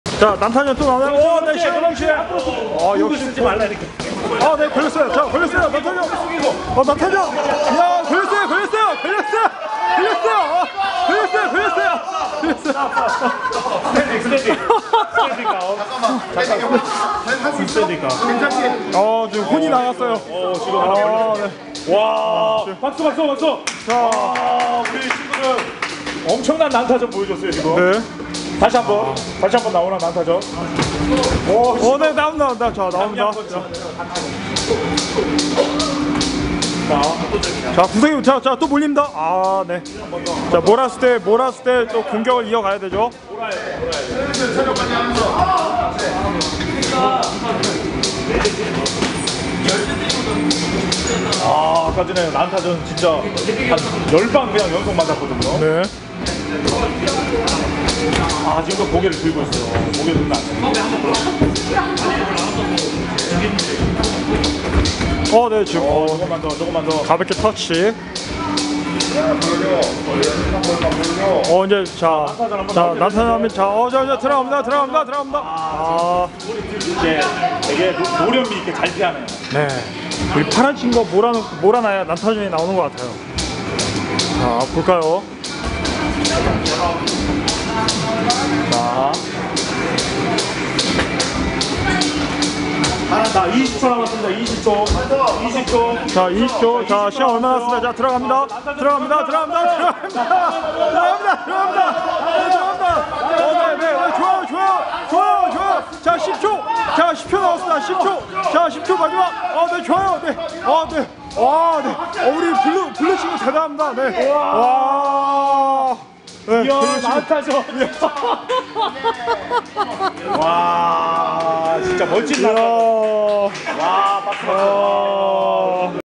고등학생들의 엄청난 난타전 스파링!! 대단해요~~ㅎㅎㅎboxing sparring
천안 홍복싱클럽 고등학생들의 엄청난 난타전 스파링!! 대단해요~~ㅎㅎㅎboxing sparring